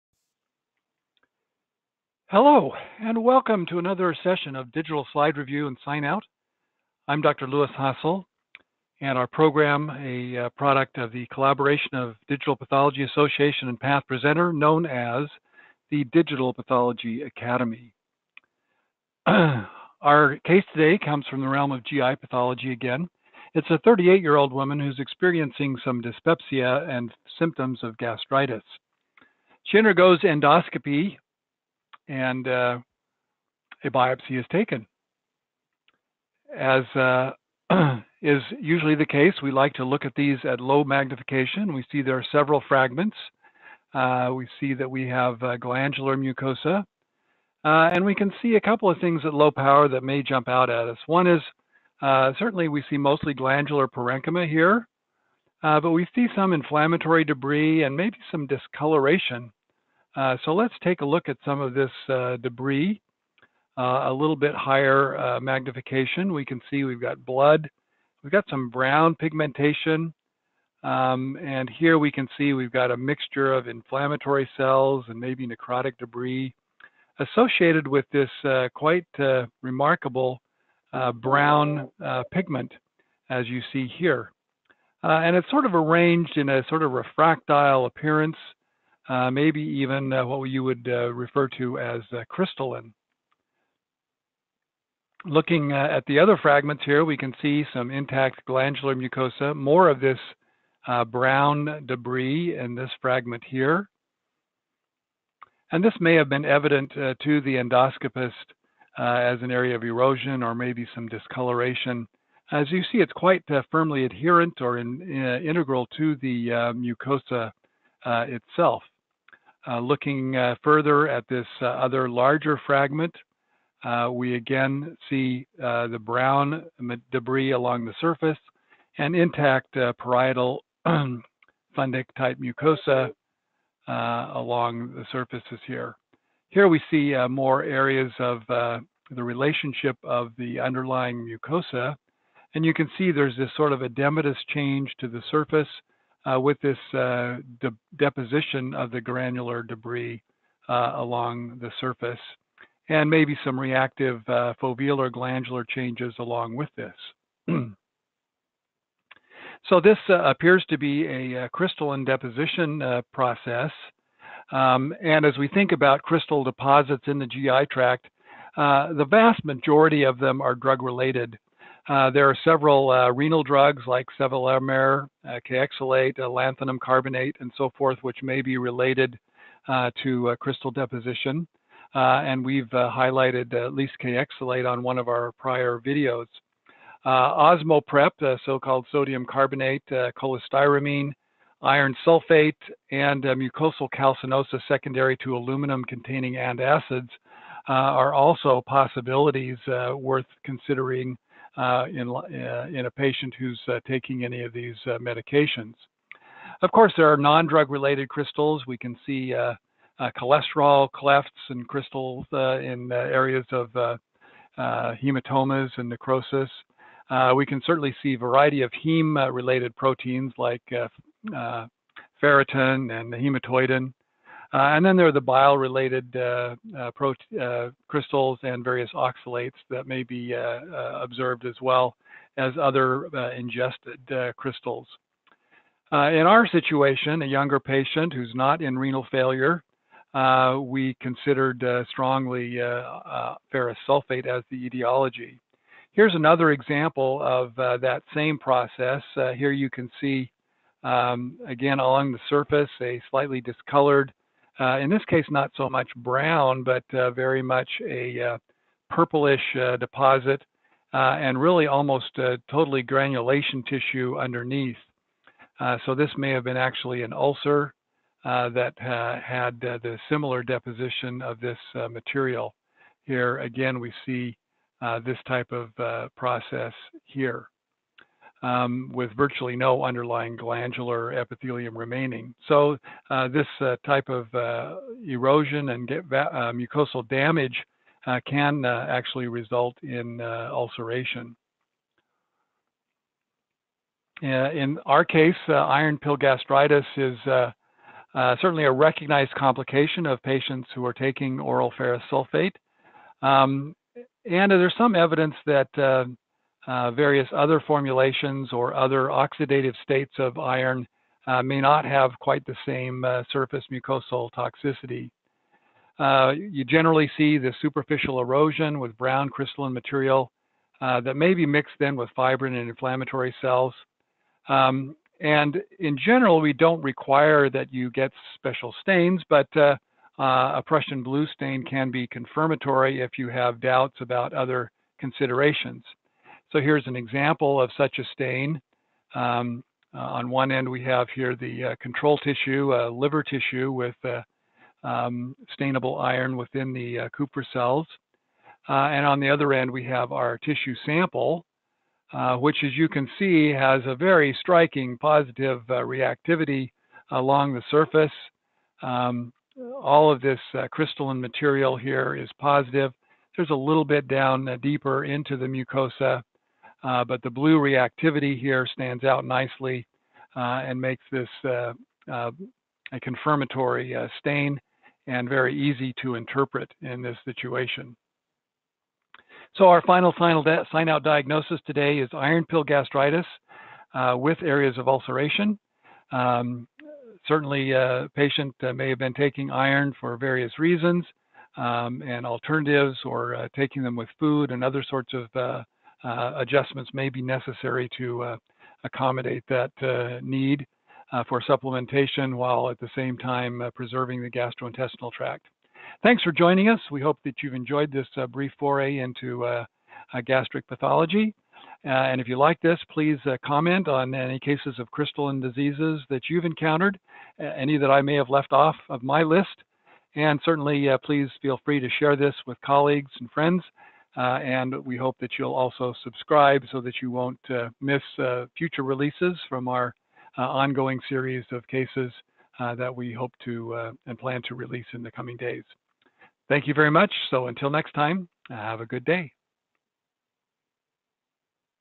Iron pill gastritis
Crystalline damage to the stomach is discussed in differential terms, and digital slides reviewed including special stain helpful in this case.

More good content available at my public, pathology-focused Facebook group: Phat Trien GPB VN